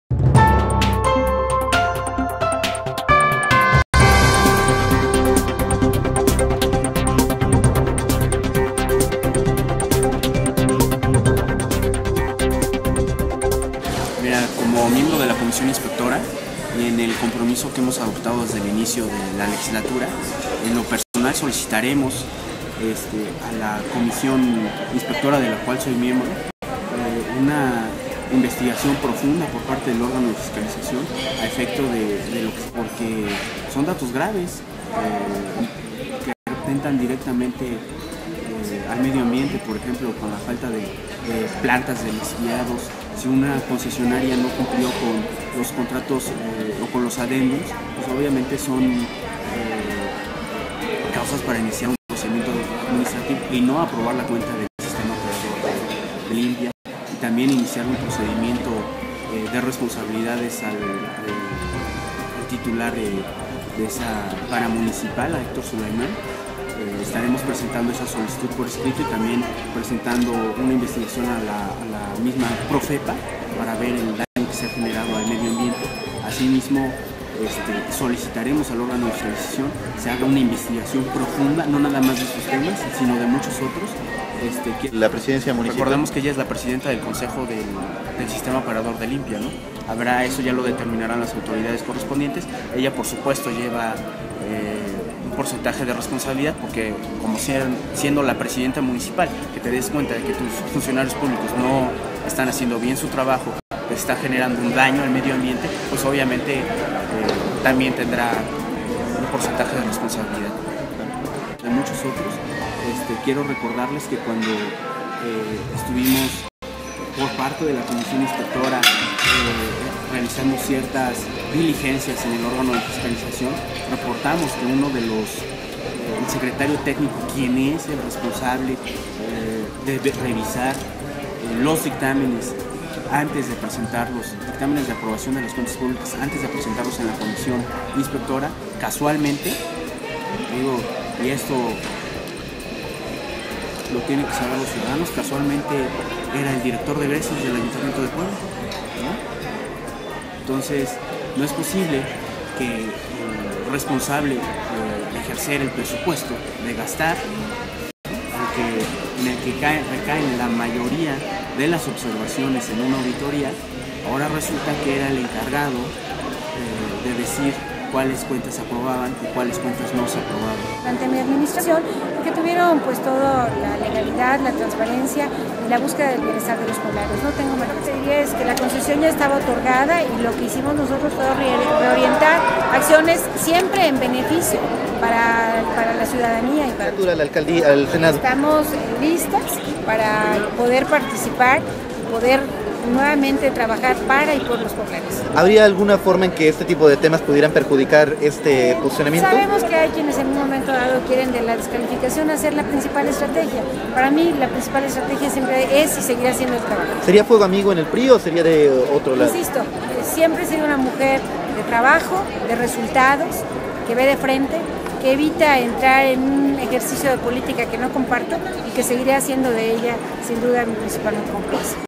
Jesús Zaldivar Anuncia Que Pedirá Investigacion vs Blanca Alcalá
El diputado local del PAN anunció que pedirá que no se apruebe la cuenta pública del Organismo Operador de Limpia y que se investigue a Héctor Sulaimán y Blanca Alcalá.